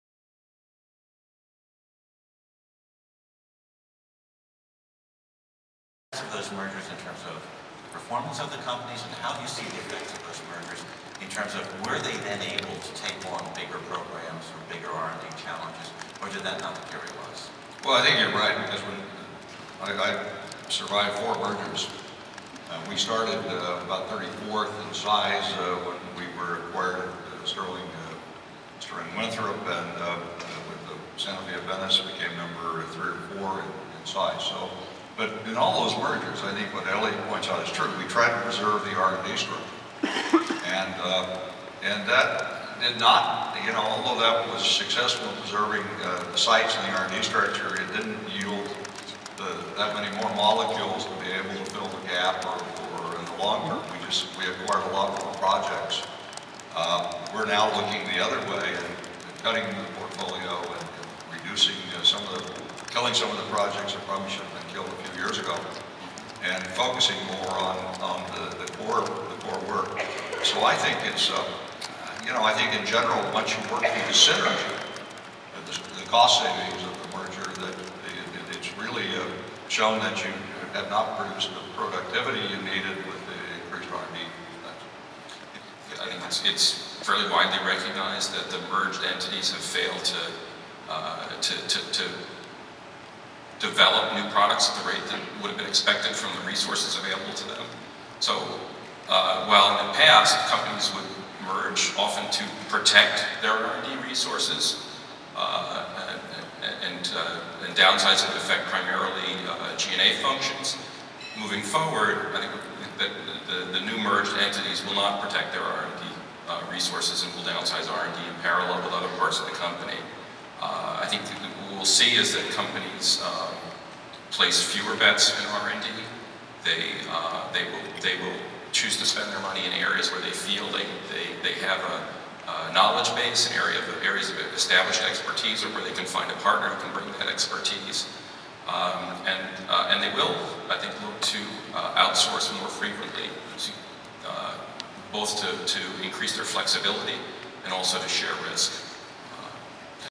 Partnerships with CROs Keynote Panel: Leading Through Change
Clips from the 18th Annual Partnerships with CROs keynote panel discussion:
Leading Through Change: Innovation and the Future of the BioPharmaceutical Industry

Moderator:
Christopher C. Gallen, MD, PhD, President & Chief Executive Officer, NEUROMED

Panelists:
Jack H. Dean, PhD, ScD (Hon.), DABT, Fellow ATS, President, U.S. Science and Medical Affairs (R&D), SANOFI-AVENTIS (Ret.)

Elliott Levy, MD, Vice President, Clinical Development, BRISTOL-MYERS SQUIBB